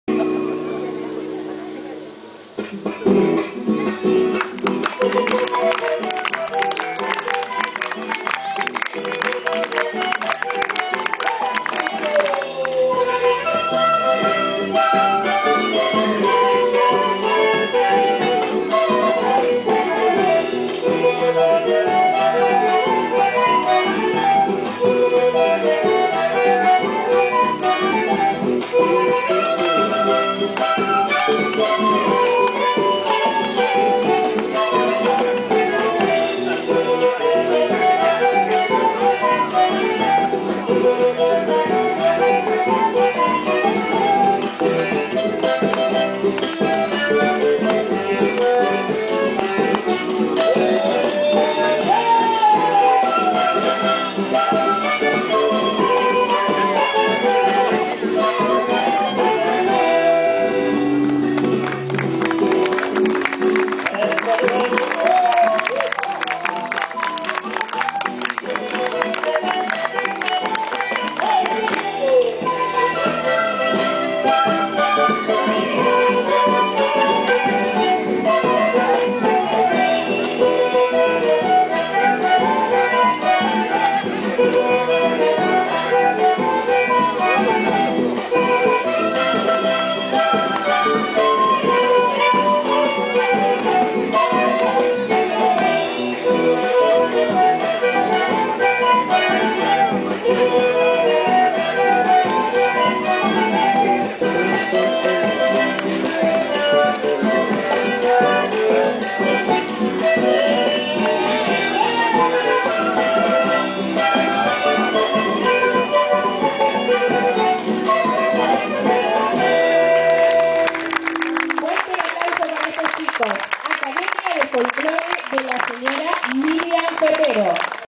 Clory Folclore popular en la plaza
Encuentro de artesanos en Villa Trinidad Raices Folcloricas representados por Clory y Gaston